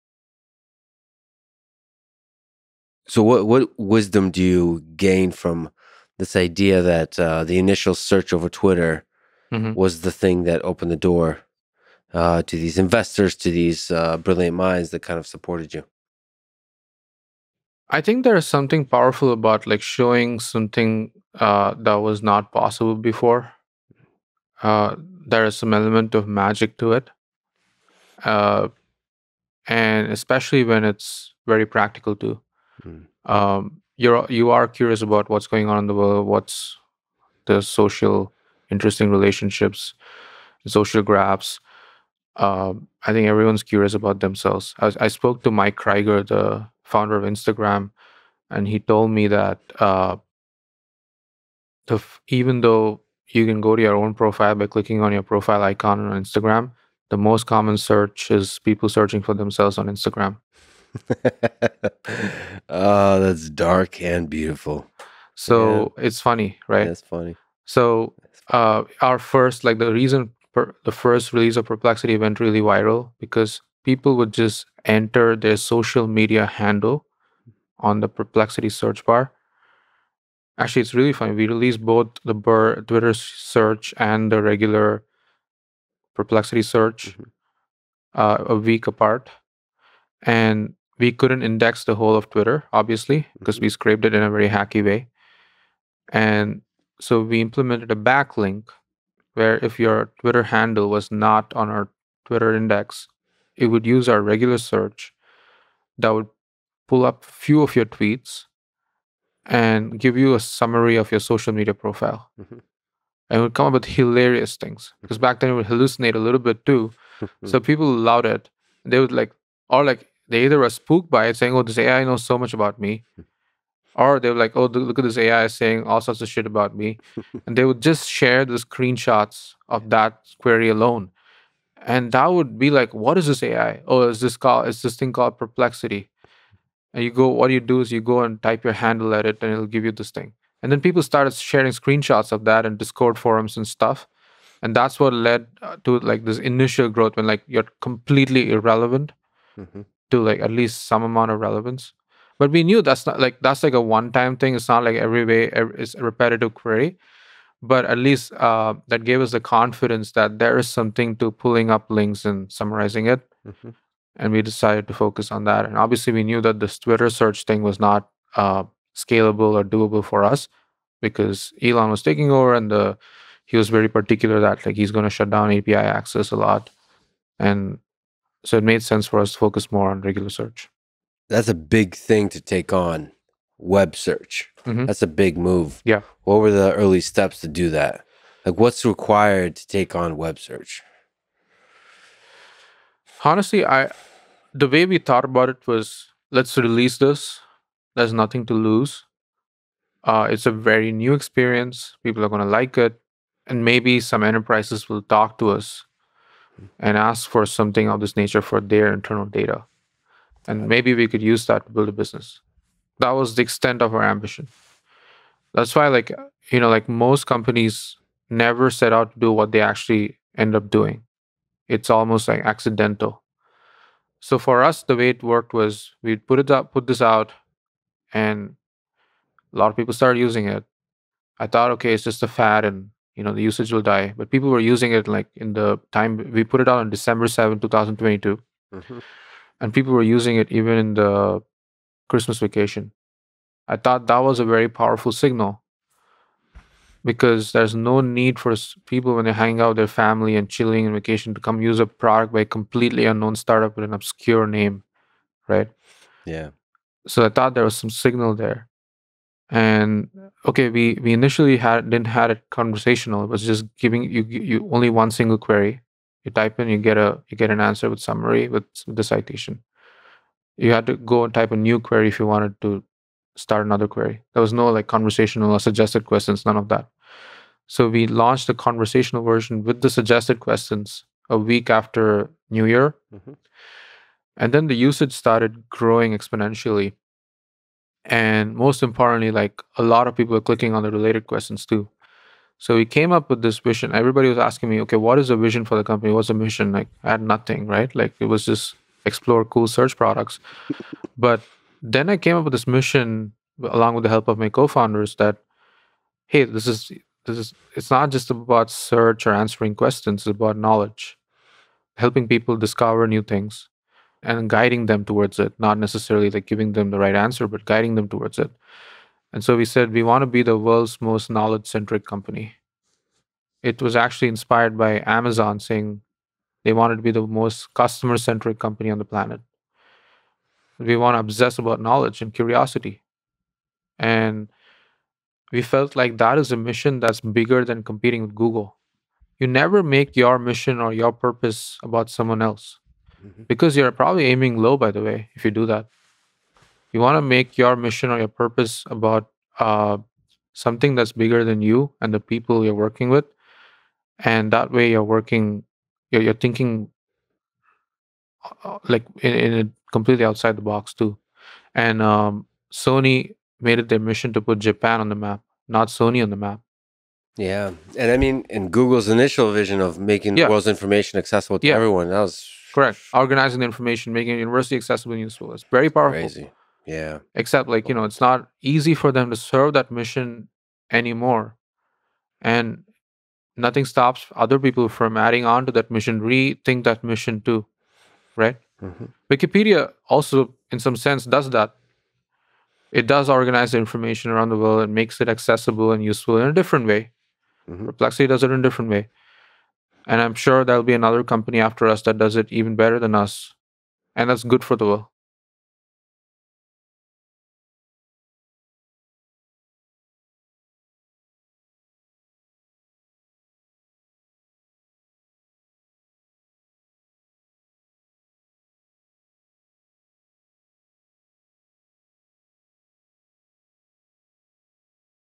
How search over Twitter opened the door for Perplexity | Aravind Srinivas and Lex Fridman
GUEST BIO:
Arvind Srinivas is CEO of Perplexity, a company that aims to revolutionize how we humans find answers to questions on the Internet.